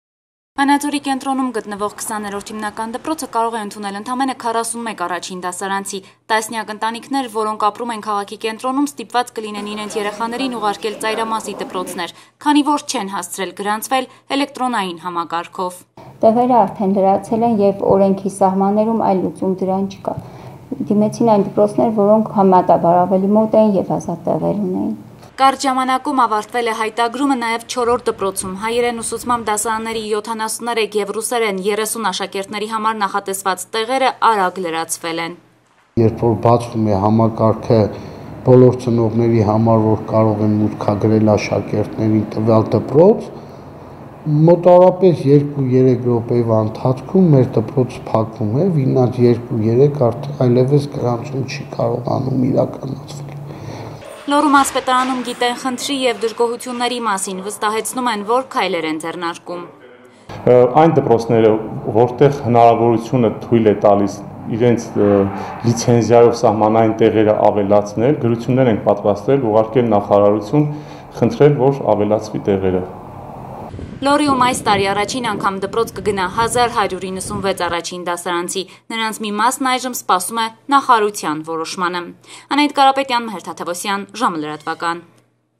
Վանաձորում առաջինդասարանցիների գրանցման էլեկտրոնային համակարգից դժգոհ են
Նոր ուսումնական տարվան ընդառաջ, Վանաձորում աշակերտների գրանցման էլկտրոնային համակարգը դժգոհություններ է առաջացրել, շատ ծնողները կանգել են փաստի առաջ: Տասնյակ ընտանիքներ, որոնք ապրում են քաղաքի կենտրոնում, այժմ ստիպված են իրենց երեխաներին ուղարկել ծայրամասի դպրոցներ, քանի որ չեն հասցրել գրանցվել: Լոռու մարզպետարանում տեղյակ են խնդրի մասին, վստահեցնում են, որ քայլեր են ձեռնարկում: